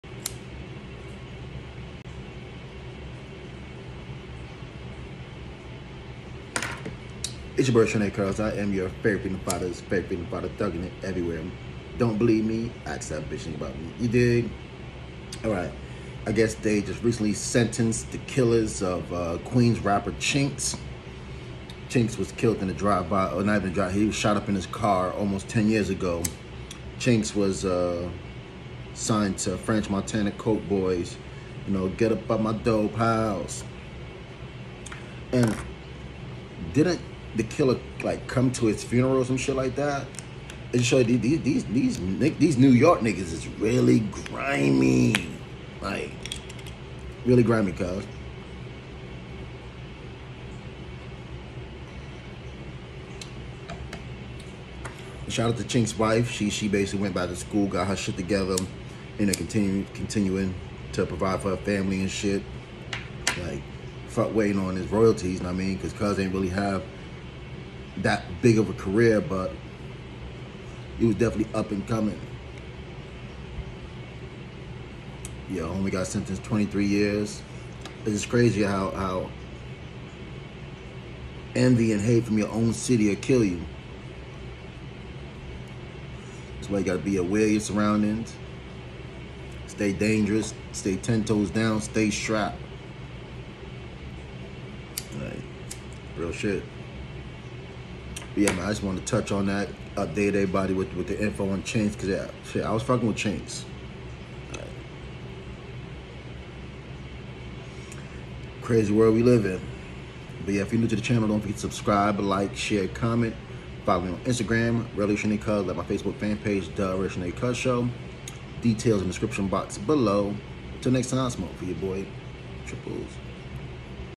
Killers Of Rapper Chinx Sentenced
FOX 5 NEWS COVERAGE

LIVE SHOW Coming back soon !!!

Look out from new project from me & Scott La Rock Jr